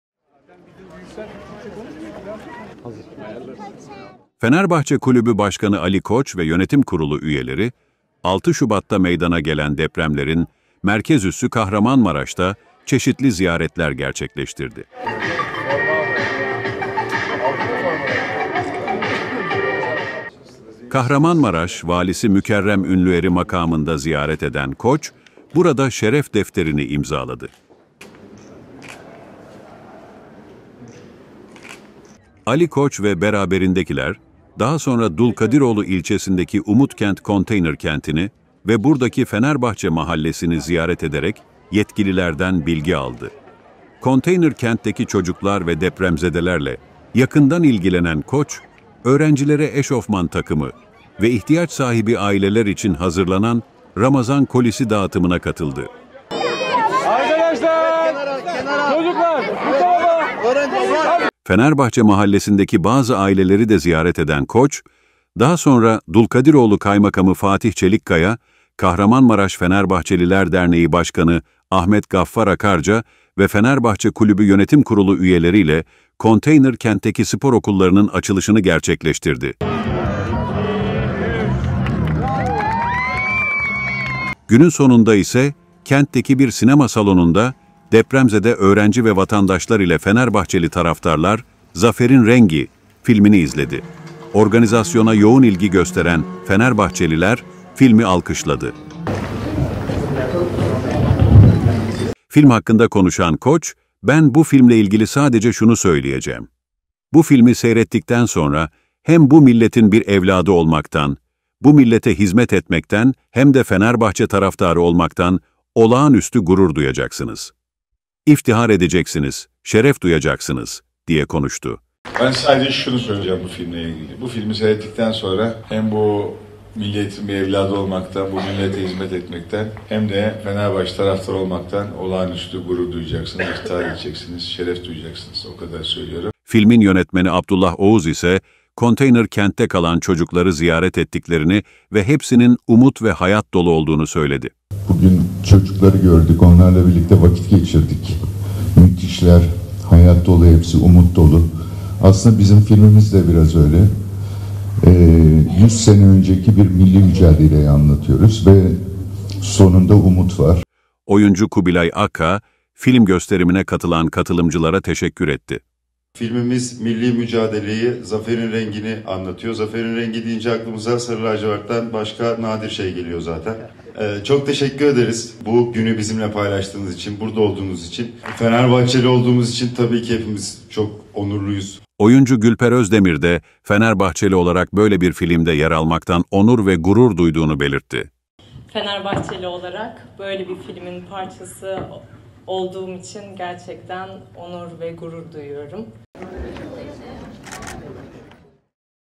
Fenerbahçe Kulübü Başkanı Koç, Kahramanmaraş'ta Ziyaretlerde Bulundu
Fenerbahçe Kulübü Başkanı Ali Koç ve yönetim kurulu üyeleri, 6 Şubat 2023'te meydana gelen depremlerin merkez üssü Kahramanmaraş'ta çeşitli ziyaretler gerçekleştirdi.
Kahramanmaraş Valisi Mükerrem Ünlüer'i makamında ziyaret eden Koç, burada Şeref Defteri'ni imzaladı.
Ali Koç ve beraberindekiler, daha sonra Dulkadiroğlu ilçesindeki Umut Kent Konteyner Kenti'ni ve buradaki Fenerbahçe Mahallesi'ni ziyaret ederek, yetkililerden bilgi aldı.
Konteyner kentteki çocuklar ve depremzedelerle yakından ilgilenen Koç, öğrencilere eşofman takımı ve ihtiyaç sahibi aileler için hazırlanan ramazan kolisi dağıtımına katıldı.

Fenerbahçe Mahallesi'ndeki bazı aileleri de ziyaret eden Koç, daha sonra Dulkadiroğlu Kaymakamı Fatih Çelikkaya, Kahramanmaraş Fenerbahçeliler Derneği Başkanı Ahmet Gaffar Akarca ve Fenerbahçe Kulübü yönetim kurulu üyeleriyle konteyner kentteki spor okullarının açılışını gerçekleştirdi.
Günün sonunda ise kentteki bir sinema salonunda depremzede öğrenci ve vatandaşlar ile Fenerbahçeli taraftarlar "Zaferin Rengi" filmini izledi.
Organizasyona yoğun ilgi gösteren Fenerbahçeliler, filmi alkışladı.

Film hakkında konuşan Koç, "Ben bu filmle ilgili sadece şunu söyleyeceğim. Bu filmi seyrettikten sonra hem bu milletin bir evladı olmaktan, bu millete hizmet etmekten hem de Fenerbahçe taraftarı olmaktan olağan üstü gurur duyacaksınız. İftihar edeceksiniz, şeref duyacaksınız." diye konuştu.
Filmin yönetmeni Abdullah Oğuz ise konteyner kentte kalan çocukları ziyaret ettiklerini ve hepsinin umut ve hayat dolu olduğunu söyledi.
Oyuncu Kubilay Aka, Film gösterimine katılan katılımcılara teşekkür etti.
Oyuncu Gülper Özdemir de Fenerbahçeli olarak böyle bir filmde yer almaktan onur ve gurur duyduğunu belirtti.